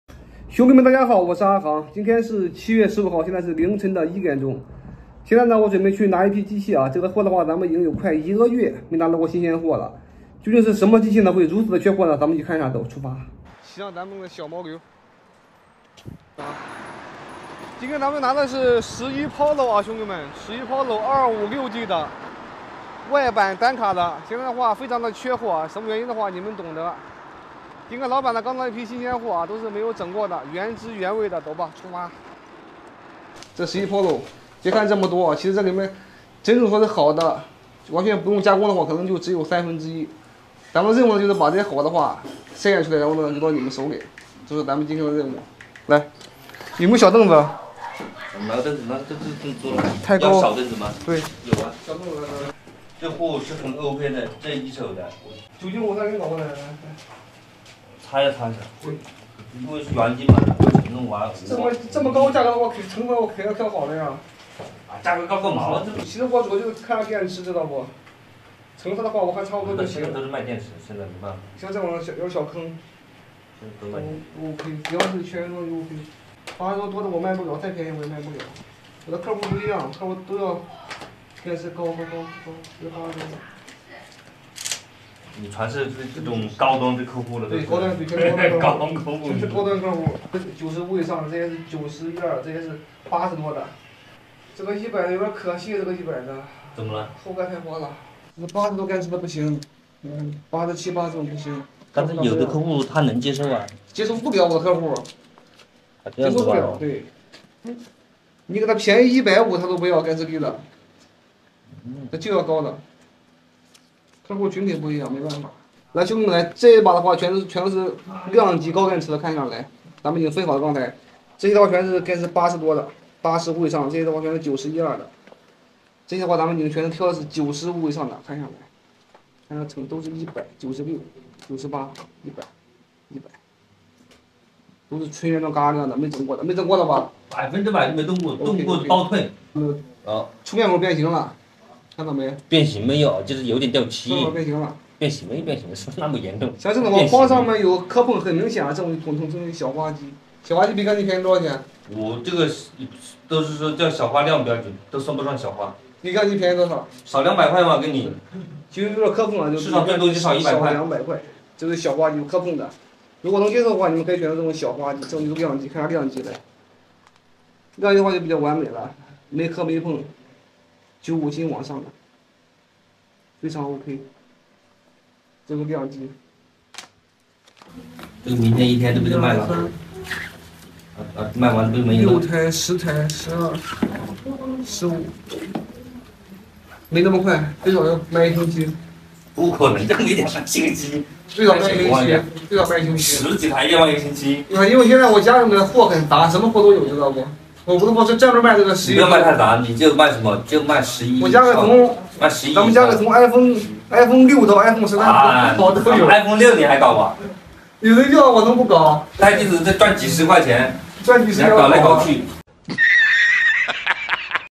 单手小旗舰 iPhone 11 pro 如今暴跌至3000元出头：果断打包几十台划算吗？
極客 blog